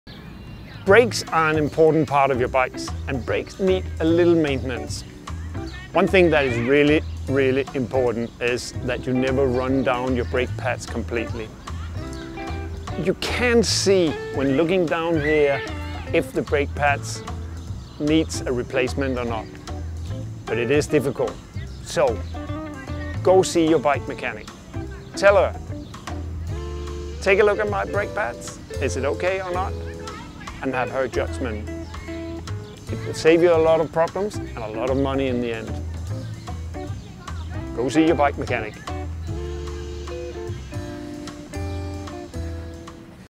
Bullitt Maintenance: Brakepads
Brakepad adjustment and keeping an eye on wear rates isn't only important for braking effectively, but can save you money on replacing disc rotors through the life of your Bullitt. Harry has some tips for us.